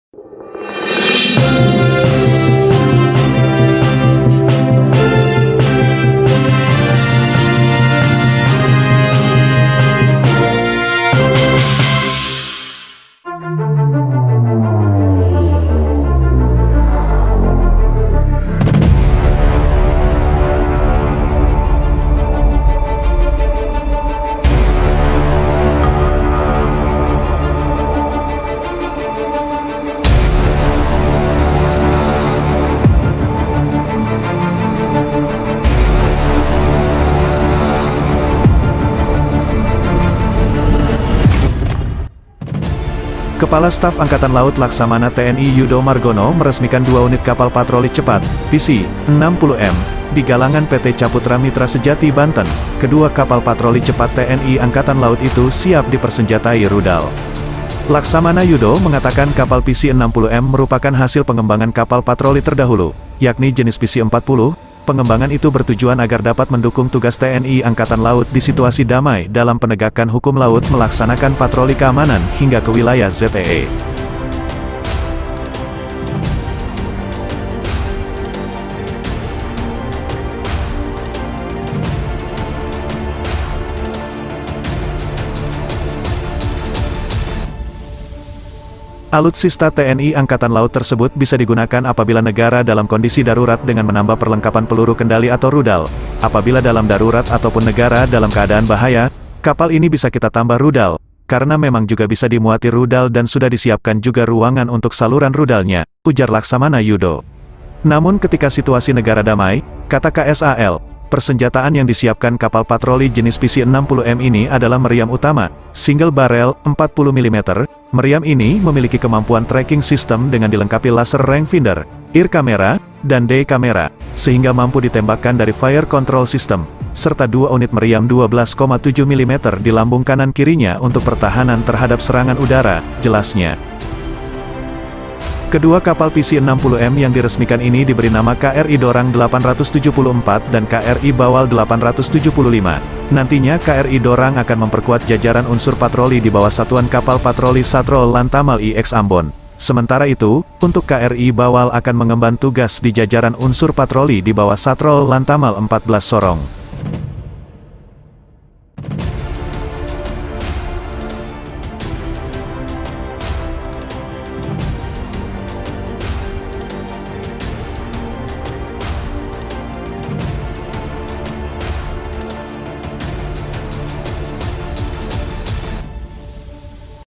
CINA RUSIA TERKEJUT SETELAH LIHAT 2 KAPAL PERANG BARU BUATAN INDONESIA
Kepala Staf Angkatan Laut (KSAL) Laksamana TNI Yudo Margono meresmikan 2 unit Kapal Patroli Cepat (PC) 60 M, di Galangan PT Caputra Mitra Sejati (CMS), Banten.

Laksamana Yudo mengatakan, Kapal PC 60 M merupakan hasil pengembangan kapal patroli terdahulu, yakni jenis PC-40. Pengembangan itu bertujuan agar dapat mendukung tugas TNI AL di situasi damai dalam penegakkan hukum laut melaksanakan patroli keamanan hingga ke wilayah ZEE.